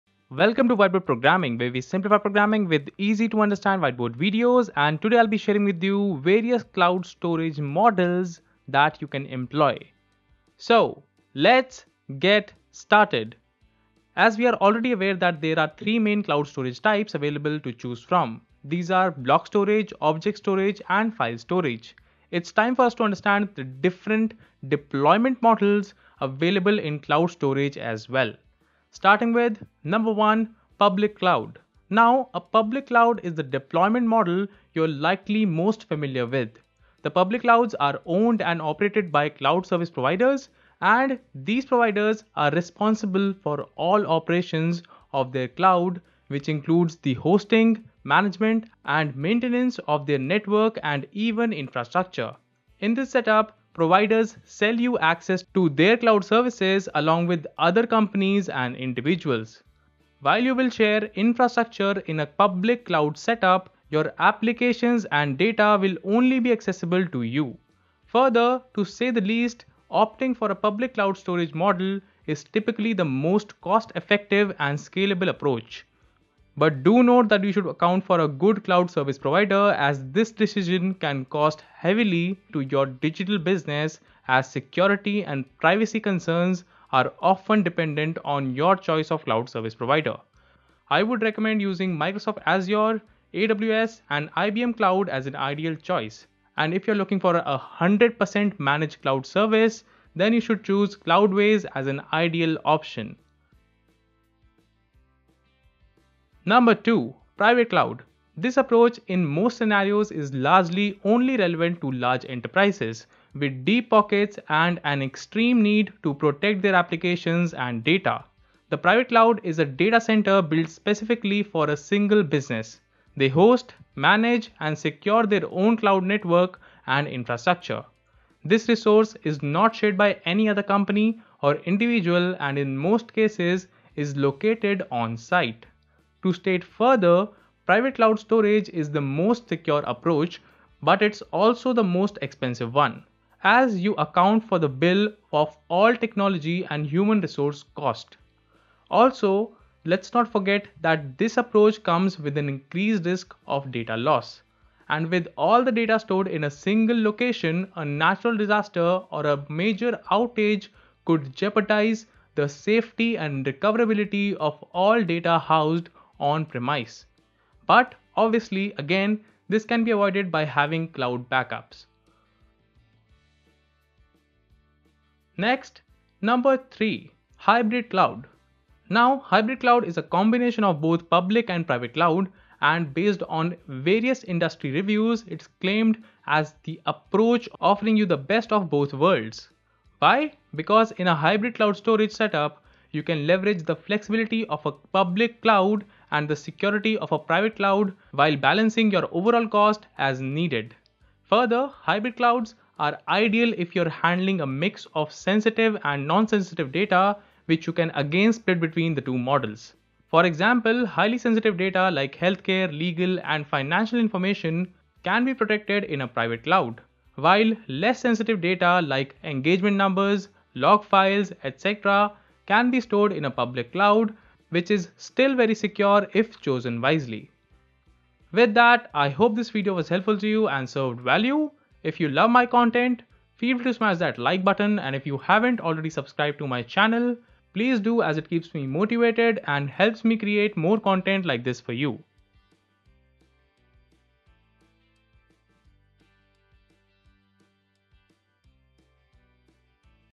Public Cloud vs Private Cloud vs Hybrid Cloud | Types of Cloud Storage Models
Supercharge your website with Kinsta's Managed Cloud Hosting

What are Cloud Storage Models? Well, Public, private, and hybrid cloud storage models are often compared based on their accessibility, scalability, security, and cost-effectiveness. Let's learn what they mean and what is the difference between public cloud vs private cloud vs hybrid cloud.

Video Walkthrough,
0:00 Introduction
0:27 What is Public Cloud
1:46 What is Private Cloud
2:55 What is Hybrid Cloud

Got feedback on this video of cloud storage models explaining public vs private vs hybrid cloud?